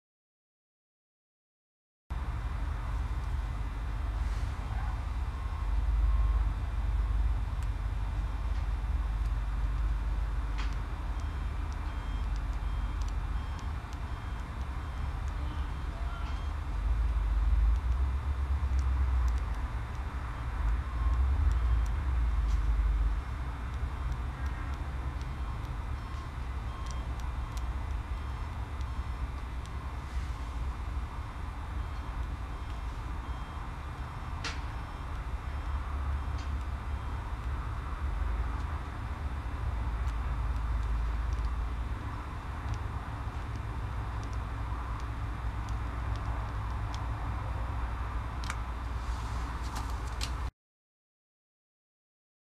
The Great Race - Arthur Ganson
An animation drawn on the edge of a paperback when I was a kid.